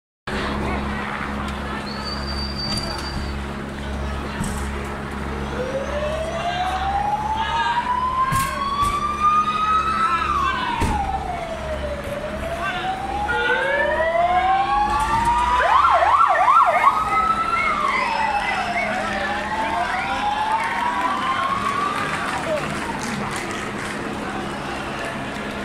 Els Mossos abandonen la part baixa de la Via Laietana després de l'última càrrega
Entre xiulets i aplaudiments (els manifestants aplaudien perquè marxaven), els antiavalots han marxat de la zona, després de l'última càrrega contra els manifestants del 21-D.